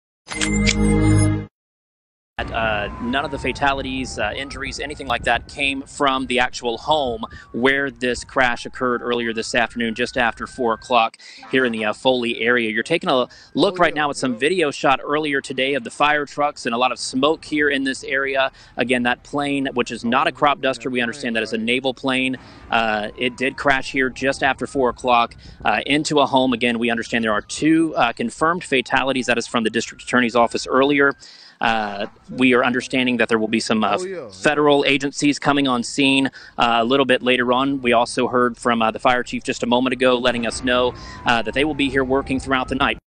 美国军机撞民宅坠毁：两名飞行员丧生，多辆汽车被破坏
2020年10月24日